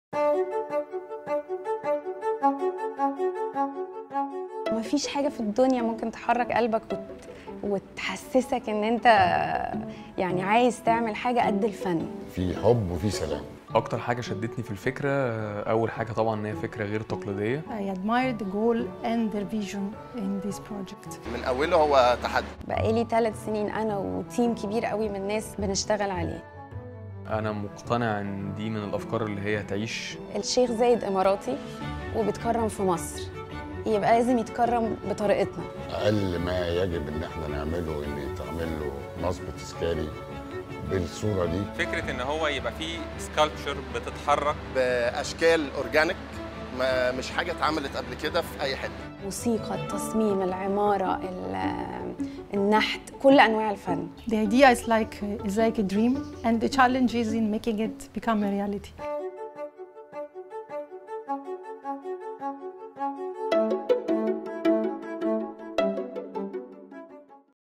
The Great Zayed Memorial ^ Trailer
تريلر مشروع فنى مبتكر بالتعاون مع مستشفى ٥٧٣٥٧ لتكريم الشيخ زايد آل نهيان، إنتظروا تفاصيل المشروع كاملاً الإسبوع المقبل.